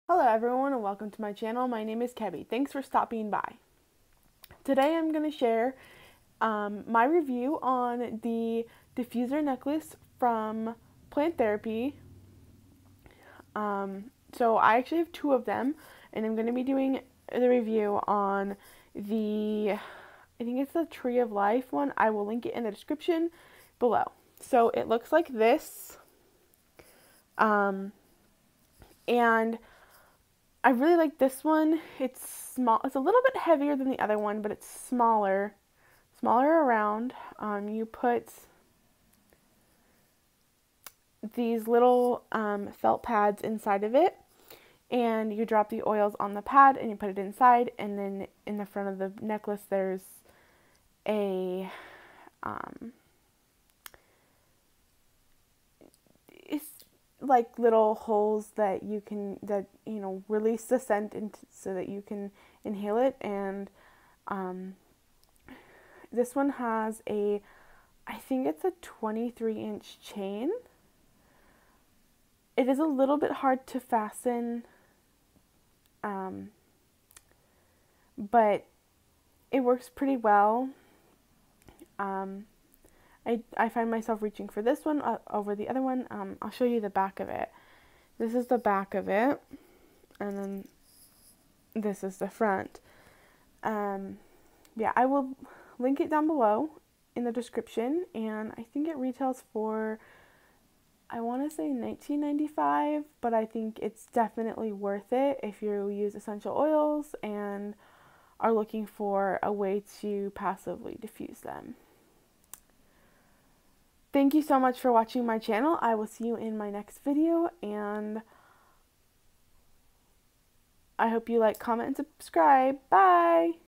Essential oil diffuser necklace review
Here’s where I share a review of the tree of life essential oil diffuser necklace from plant therapy. You can purchase this necklace at the following